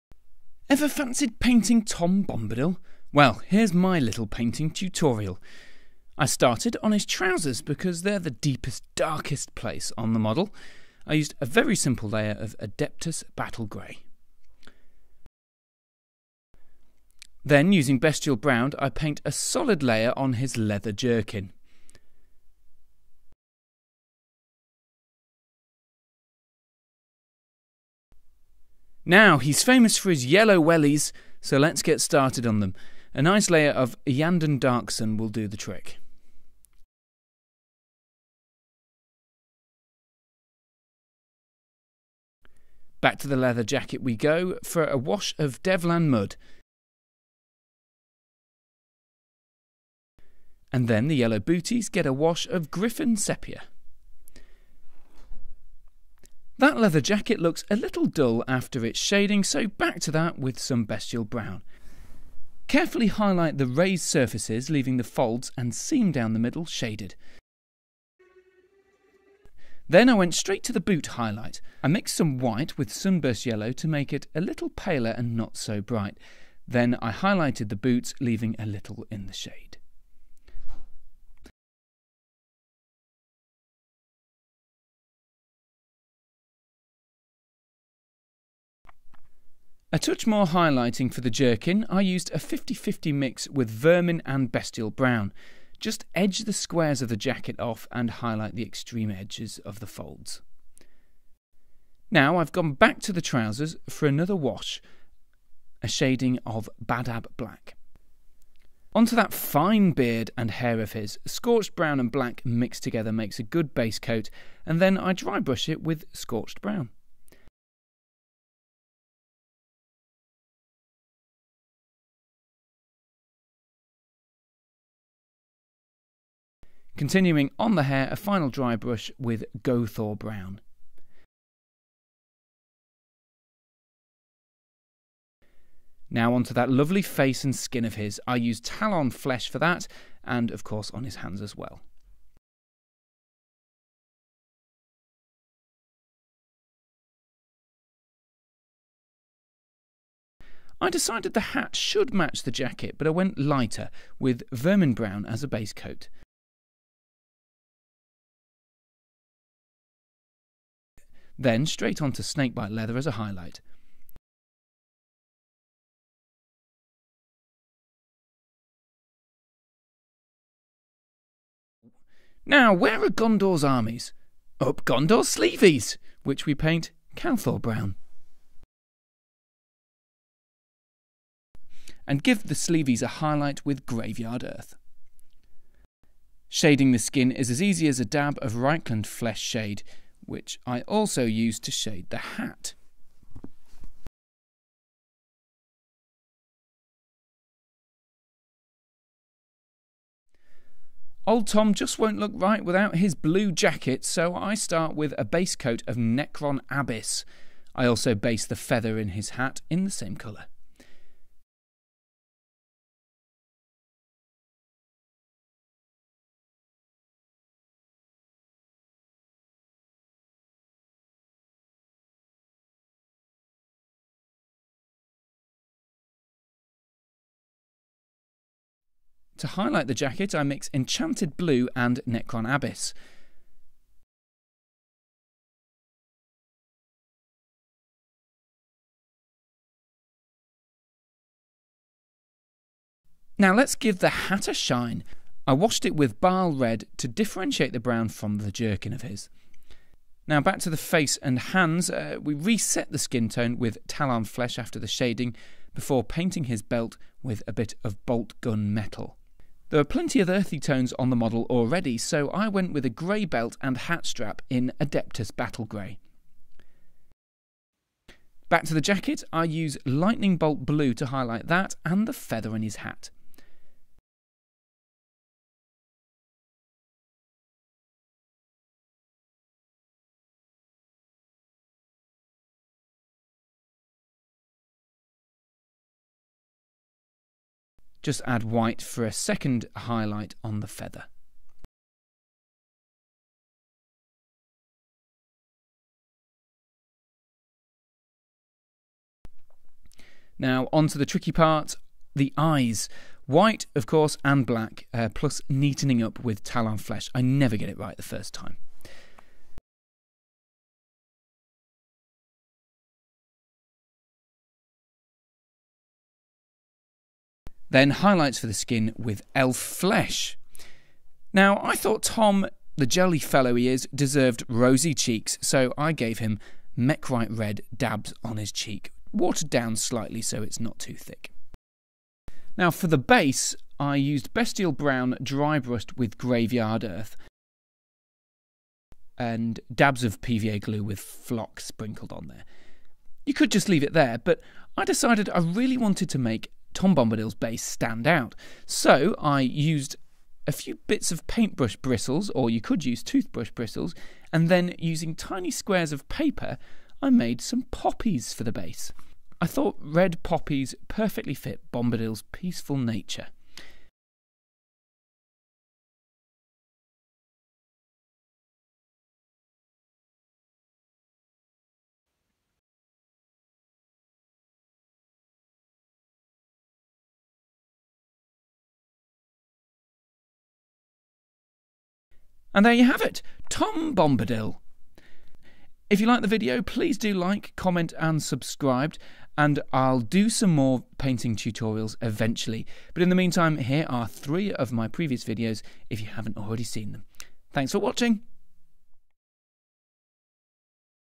How to paint Tom Bombadil for Lord of the Rings Warhammer
A painting guide for Tom Bombadil from the Middle Earth Strategy Battle Game.